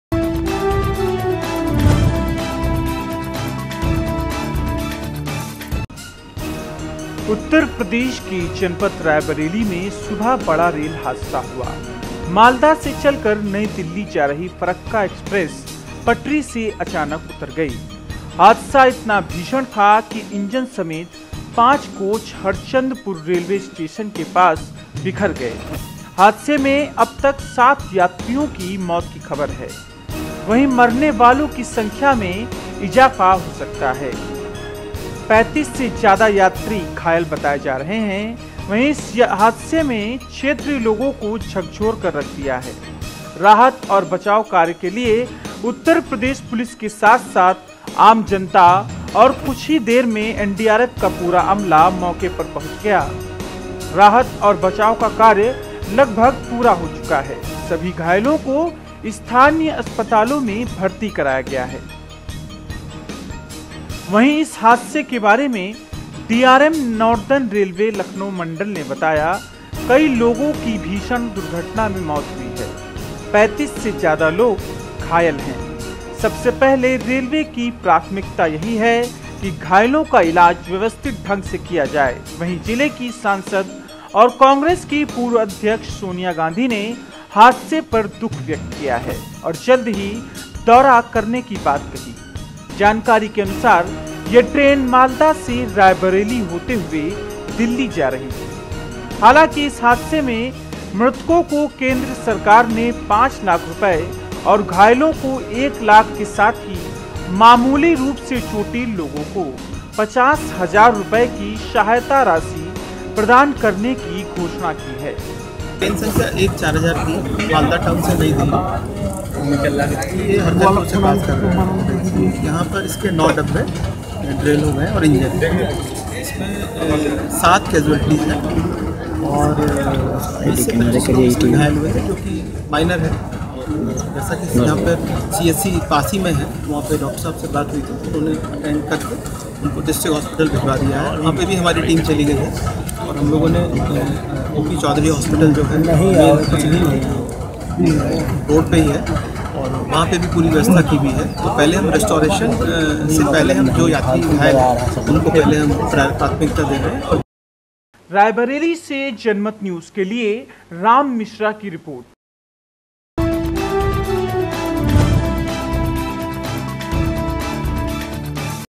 इस रेल हादसे ने निगल ली कई मासूमो की जिंदगियां.....
उत्तर प्रदेश के जनपद रायबरेली में सुबह मालदा से चलकर नई दिल्ली जा रही फरक्का एक्सप्रेस ई रेल हो गई हादसा इतना भीषण था की इंजन समेत पांच कोच हरचंदपुर रेलवे स्टेशन से लगभग 300 मीटर पूरब में बिखर गए लगभग 5:45 पर हुए इस हादसे में क्षेत्रीय लोगों को झकझोर कर रख दिया है सरकारी आंकड़ों के हिसाब से 6 लोगों की मौत की पुष्टि हुई है तथा 35 लोगों के घायल होने की संभावना है। राहत और बचाव कार्य के लिए उत्तर प्रदेश पुलिस के साथ-साथ आम जनता व कुछ ही देर में एनडीआरफ का पूरा जमला पहुंच गया। राहत और बचाव का कार्य लगभग पूरा हो चुका है सभी घायलों को स्थानीय अस्पतालों में भर्ती कराया गया